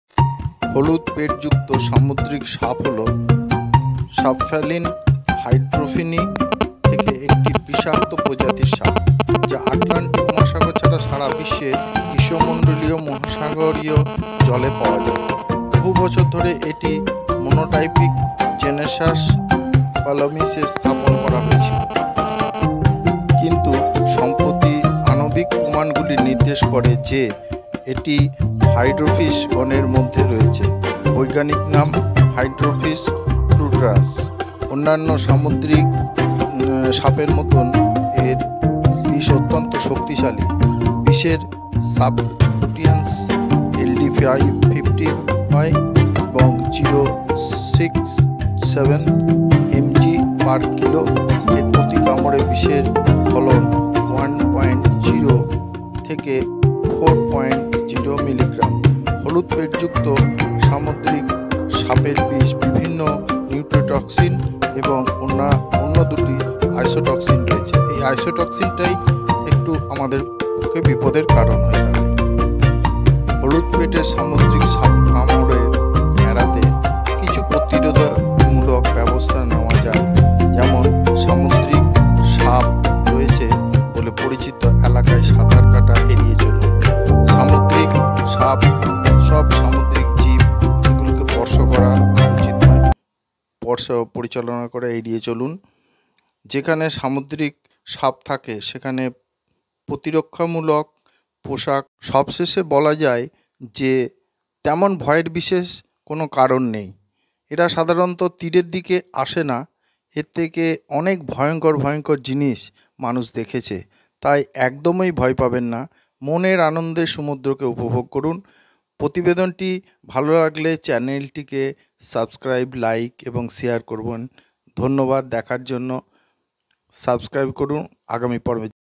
Yellow belly |पीला बोलोइड |হলুদ পেট
This is a dangrous  sea snake The yellow-bellied sea snake is a venomous species of snake from the subfamily Hydrophinae found in tropical oceanic waters around the world except the Atlantic Ocean.  For many years, it was placed in the monotypic genus Pelamis, but recent molecular evidence indicates that it belongs to the genus Hydrophys.
 Scientific name: Hydrophis platurus

 poison  Like other sea snakes, the venom of this species is very strong.  The subcutaneous LD 50 of the venom is 0.067 mg/kg and the venom yield is 1.0–4.0 mg per bite.  Yellow-bellied sea snake venom contains several neurotoxins and two other isotoxins.

 According to reports, this snake is very aggressive in biting.  Antivenom resistance does not affect its bite.  A person's bite is certain to cause paralysis as well as death.  The scientific name of this yellow-bellied snake is 'Pelamis platurus'.

 Preventive measures to avoid yellow-bellied sea snake bites

  Avoid swimming in areas known to harbor sea snakes.

  Avoid touching or handling marine life, including sea snakes.

  Wear protective clothing, such as a wetsuit or dive skin, where sea snakes are present.

After all it can be said that there is no particular reason for such fear. They do not usually come to arrows. People have seen many more terrible things than this.  So don't be afraid at all.  Enjoy the sea to your heart's content.
Thanks to all who help me making this video for music & pictures.